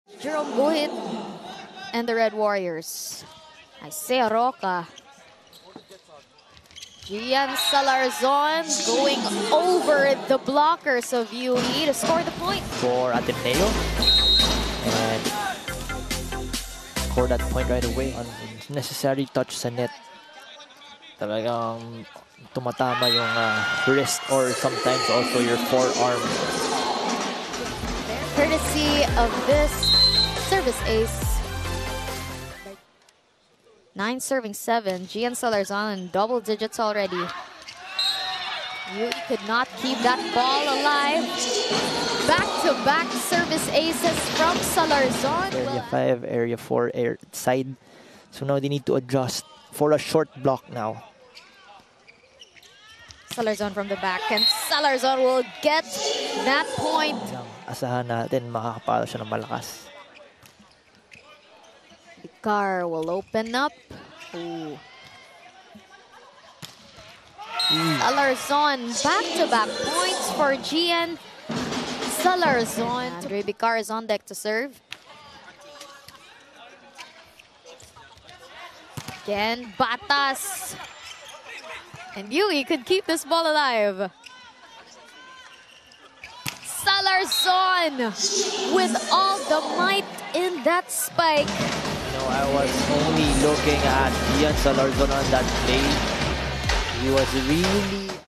ADMU's Salarzon ACTIVATES vs UE 💪 | UAAP SEASON 86 MEN'S VOLLEYBALL
JIAN SIYA MAGALING! 💪

SIX FIERY ATTACKS from Jian Salarzon sends the Ateneo Blue Eagles into a third-set advantage against the hot-handed UE Red Warriors!